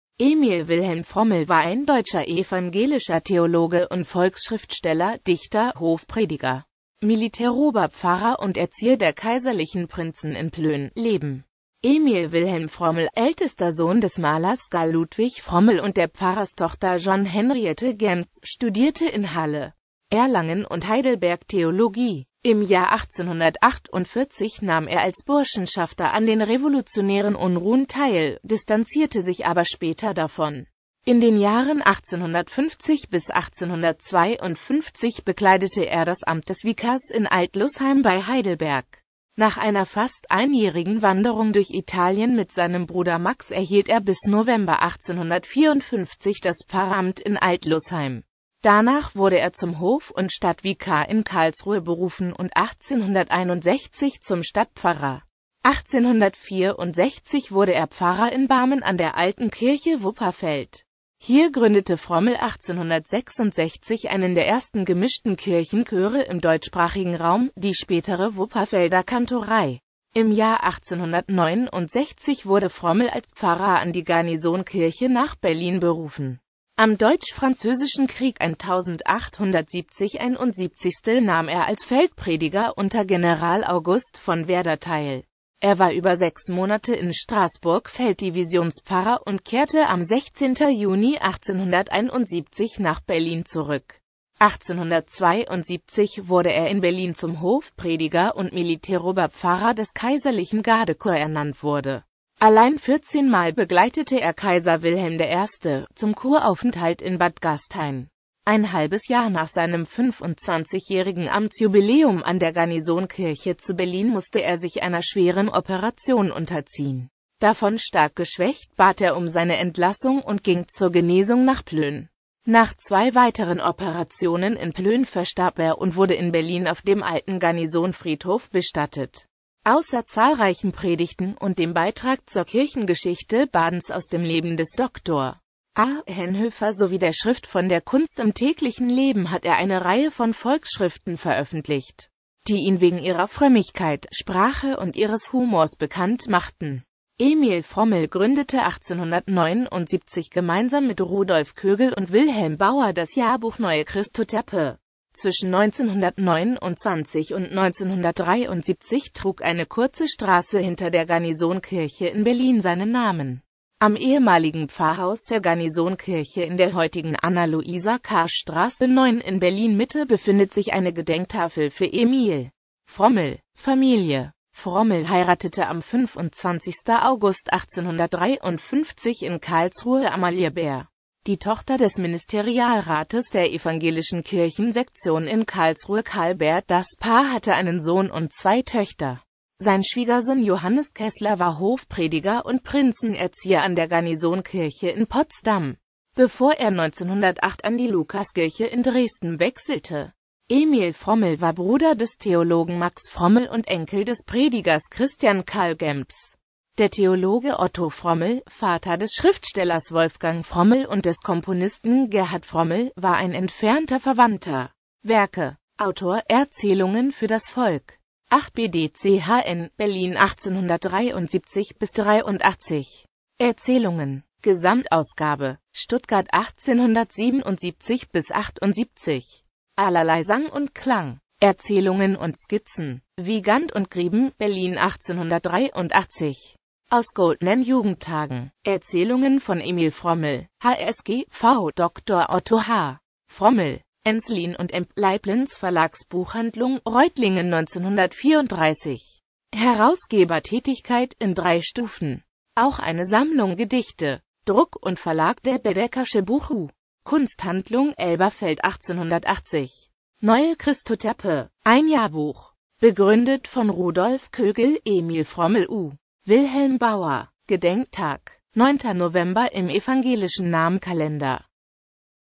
Emil Frommel
Vielen Dank für Ihre Unterstützung:

Emil Wilhelm Frommel war ein deutscher evangelischer Theologe und Volksschriftsteller, Dichter, Hofprediger, Militäroberpfarrer und Erzieher der kaiserlichen Prinzen in Plön.

Bild-Copyright-Informationen
Urheber Info: (unknown);